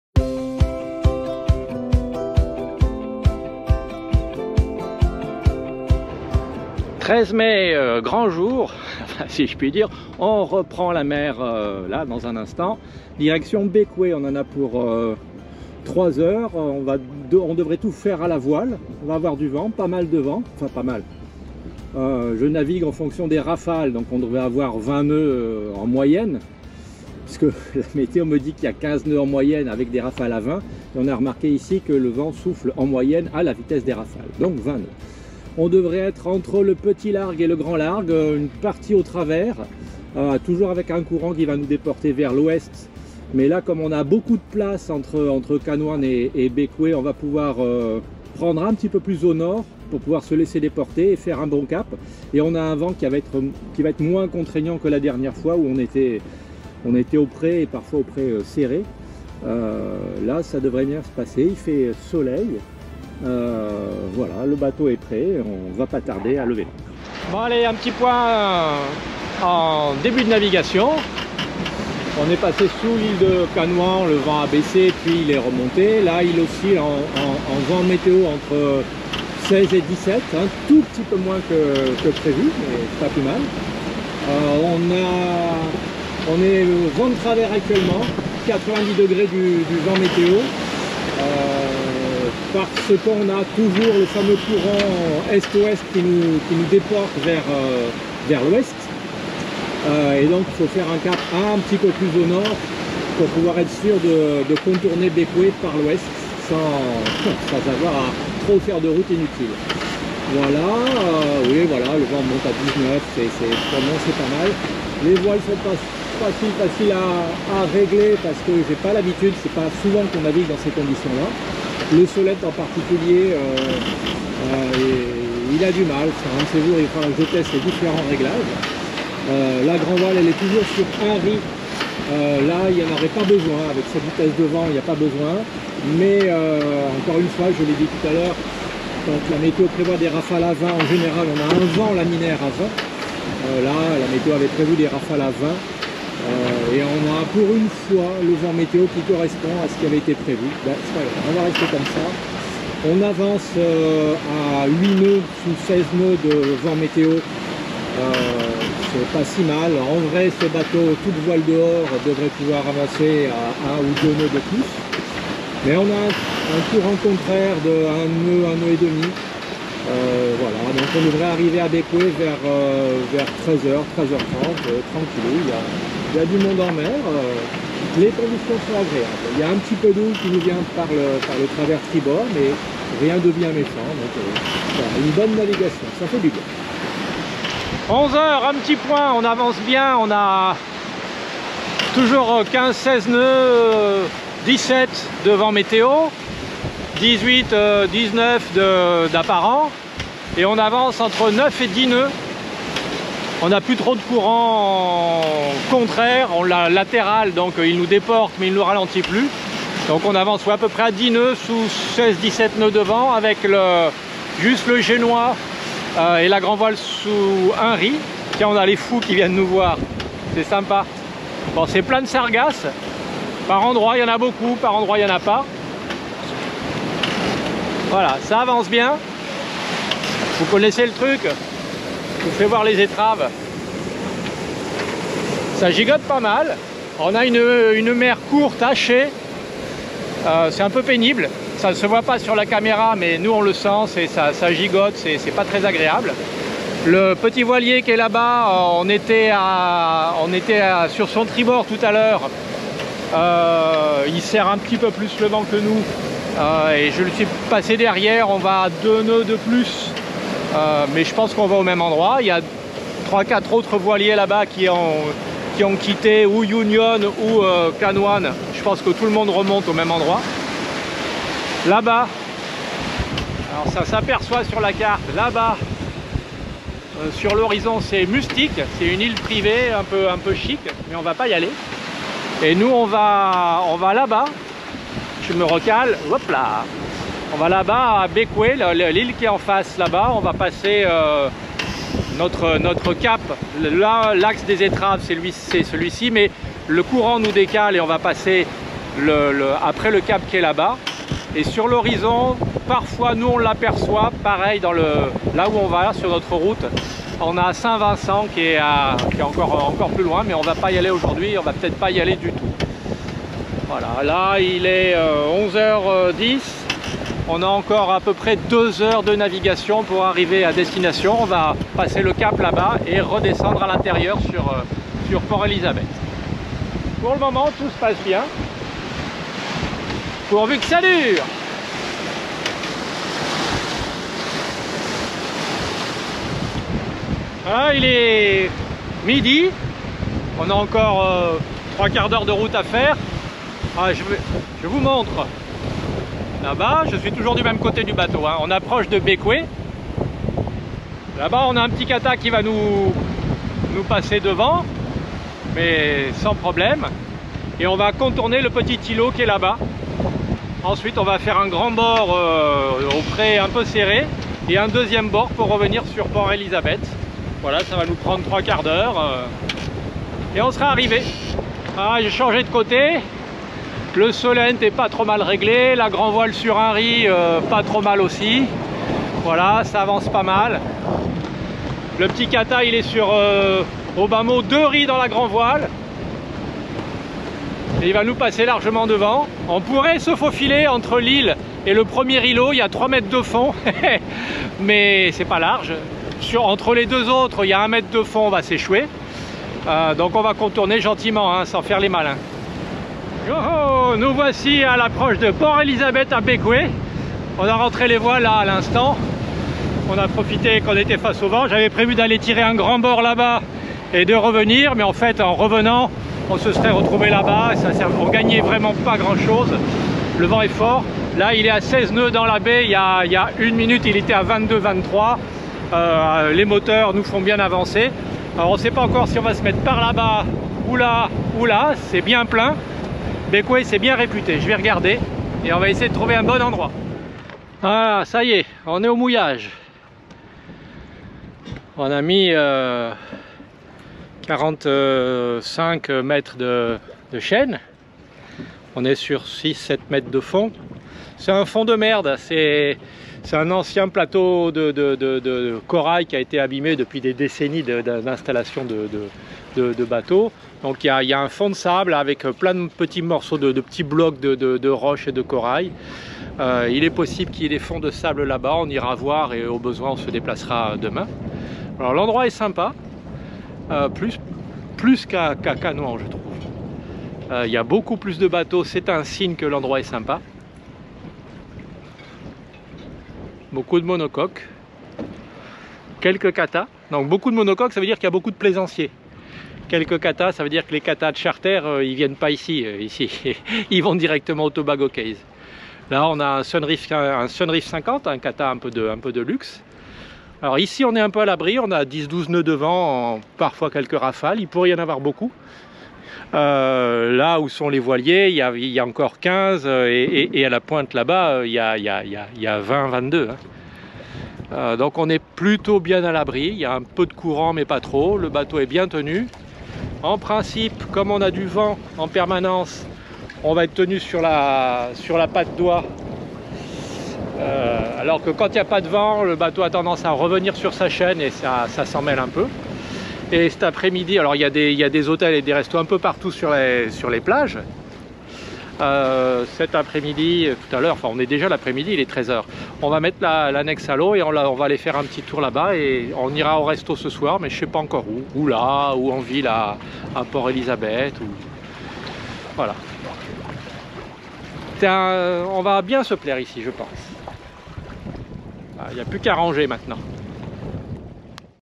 #79 - Petite navigation tonique entre Canouan et Bequia
Un peu moins de trois heures, c’est ce qu’il nous a fallu pour rejoindre Bequia à partir de Canouan, sous voiles. Une navigation qui s’est faite encore une fois y près ou au travers,sous 20 noeuds de vent, à une bonne vitesse. Tonique sans être sportif.
Au premiers regards, Bequia semble être une destination prisée et agréable…